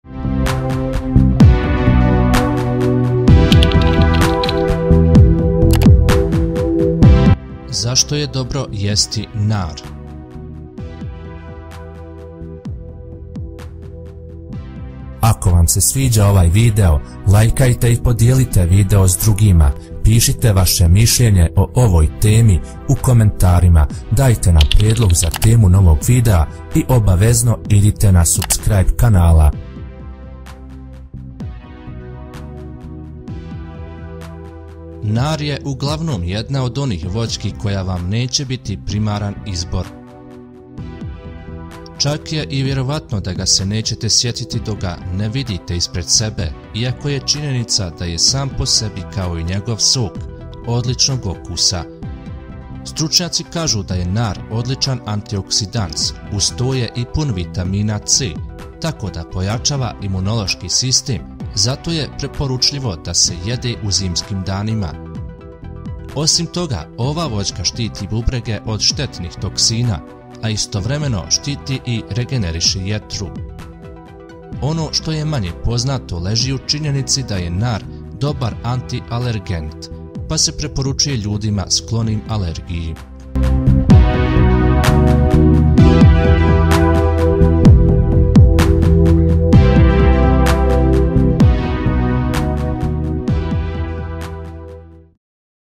Zašto je dobro jesti nar?
Nar je uglavnom jedna od onih voćki koja vam neće biti primaran izbor, čak je i verovatno da ga se nećete setiti dok ga ne vidite ispred sebe, iako je činjenica da je sam po sebi kao i njegov sok odličnog ukusa.
Stručnjaci kažu da je nar odličan antioksidans. Uz to je i pun vitamina C, tako da pojačava imunološki sistem, zato je preporučljivo da se jede u zimskim danima.
Osim toga ova voćka štiti bubrege od štetnih toksina, a istovremeno štiti i regeneriše jetru. Ono što je manje poznato leži u činjenici da je nar dobar antialergent, pa se preporučuje ljudima sklonim alergiji.